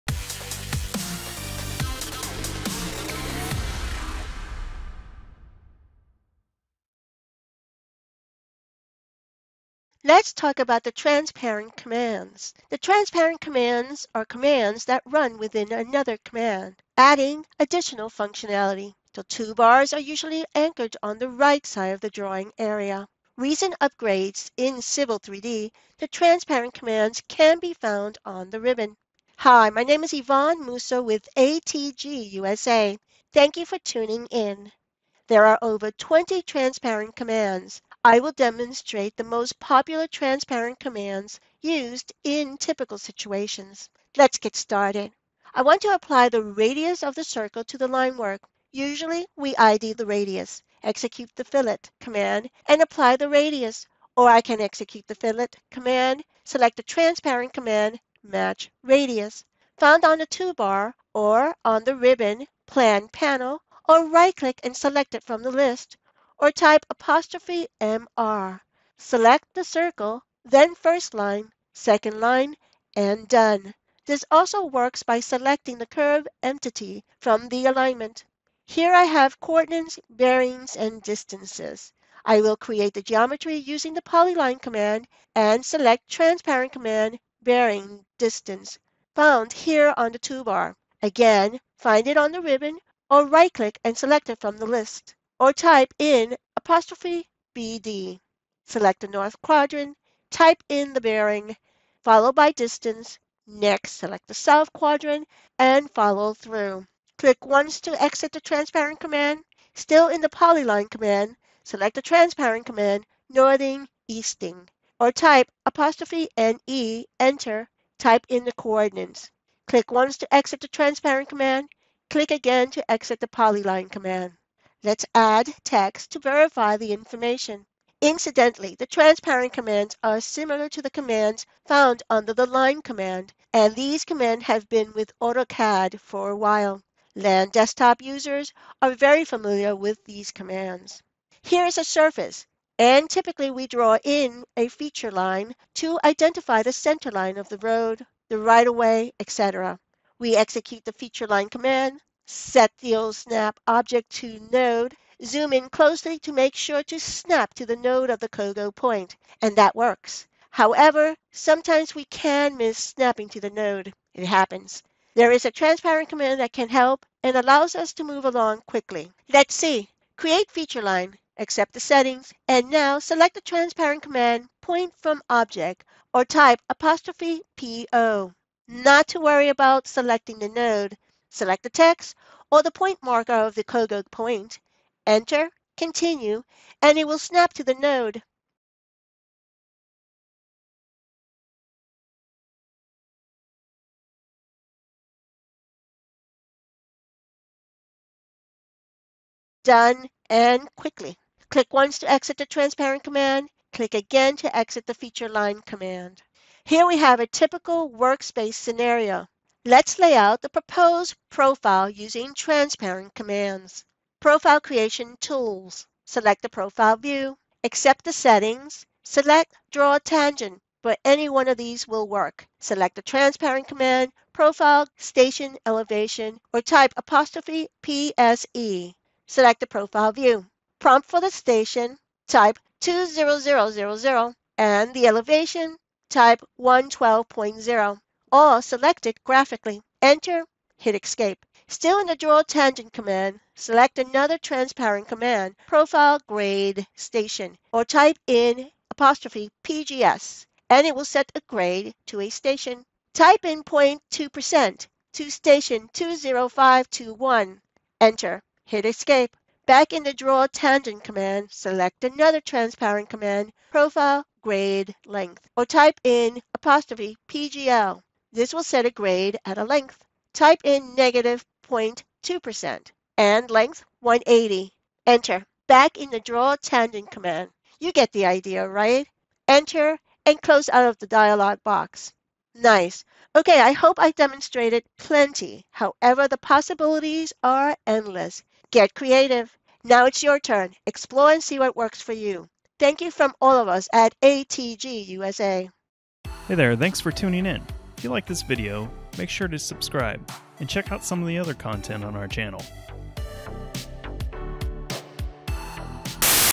Popular Transparent Commands in Civil 3D
In this video, we'll show you how to find transparent command and how to use them in many situations, such as, profile and plan designing.